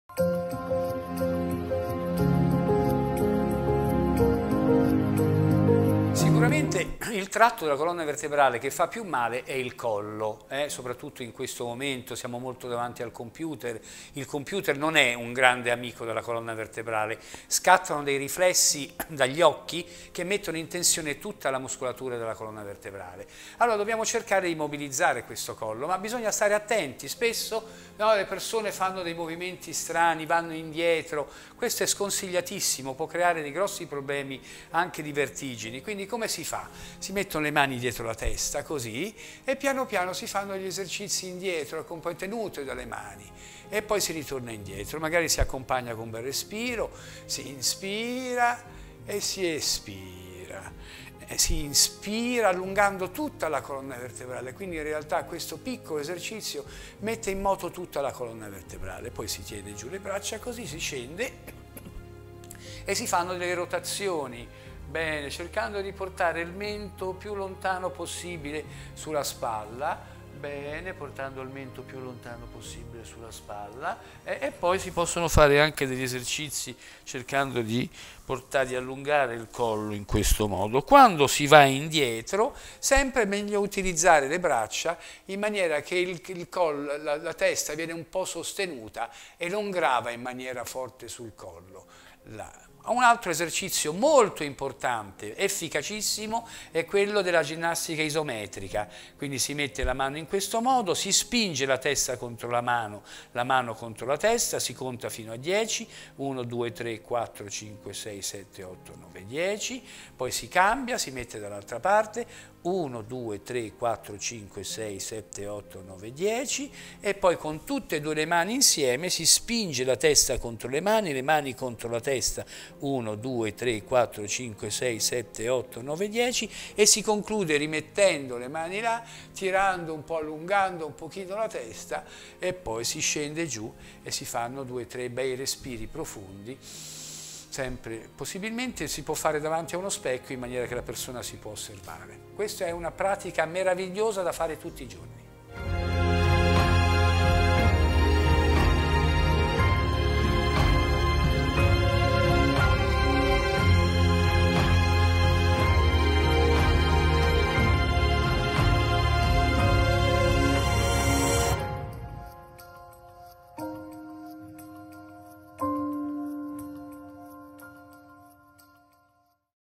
Esercizi per il collo e ginnastica isometrica per combattere la cervicale
Uno dei segmenti della schiena che spesso fa più male è quello cervicale. Esercizi di rotazione guidati e corretta respirazione aiutano a mettere in moto e allungare la colonna vertebrale. Anche la ginnastica isometrica è uno strumento efficacissimo per mobilizzare il tratto cervicale.
Fulvio Palombini, reumatologo e coordinatore del centro di Fisioterapia e riabilitazione del Poliambulatorio Apollodoro, mostra utili esercizi per il collo da fare tutti i giorni, se possibile davanti a uno specchio.